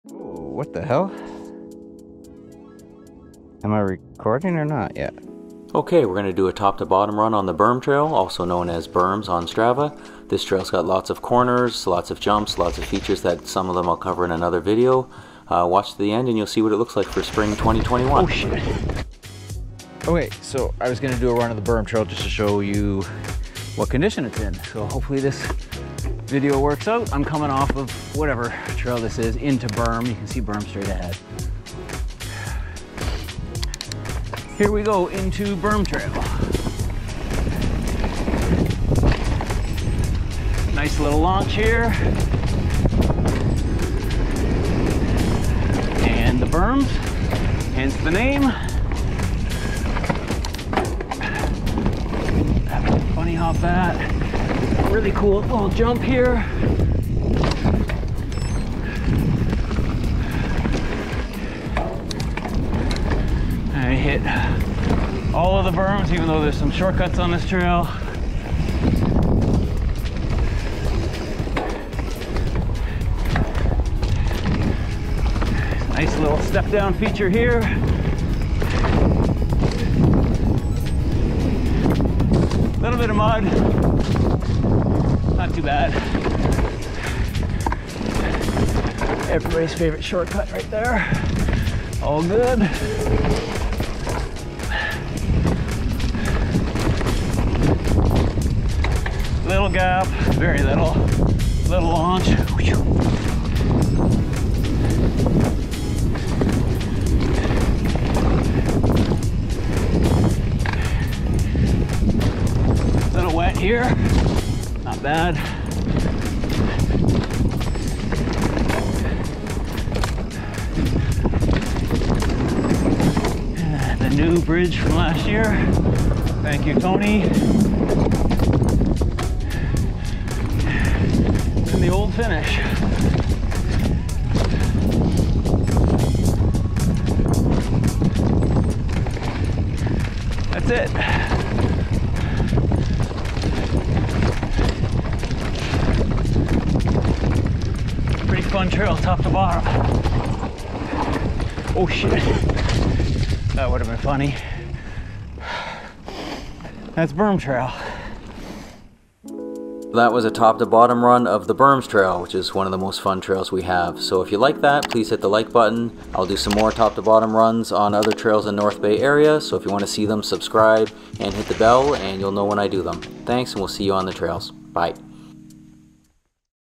Berms - T2B TRAIL RIDE | MTB in North Bay, Ontario
I'm hoping this video show riders the fun lines on 'The Berms' trail here in North Bay, ON and also how awesome the early season conditions are. Things are drying up quickly and as a result the trail is running really well. Enjoy!

The Berms trail and most North Bay area trails can be found on the Pinkbike app called 'Trailforks'. This trail is part of the Laurentian Ski Hill Escarpment network. I will cover other trails at the Campus/Monastery trail network and the Northshore Rd network later this season.

Trail Courtesy Note : During the pandemic we are seeing way more users accessing the North Bay area trail networks resulting in numerous mid-ride interactions with hikers, dog walkers, snowshoers, etc. PLEASE be courteous and friendly to ANYONE you meet on the trails and represent mountain bikers as the helpful and responsible stewards of the trails that we are. Thank you!

My Strava account is Flyin Dutchman if you do the Strava thing.

Thanks!